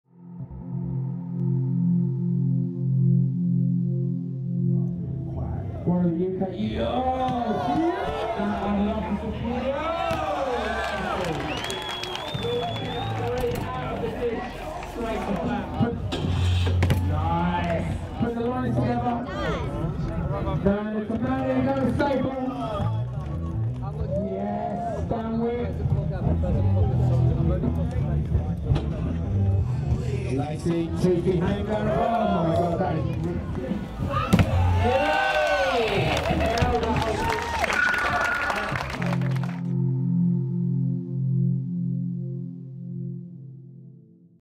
NASS 2011 - 1st Place BMX Street Spots Dan Lacey
Dan Lacey's winning run at the Relentless Energy Drink Street Spots from NASS 2011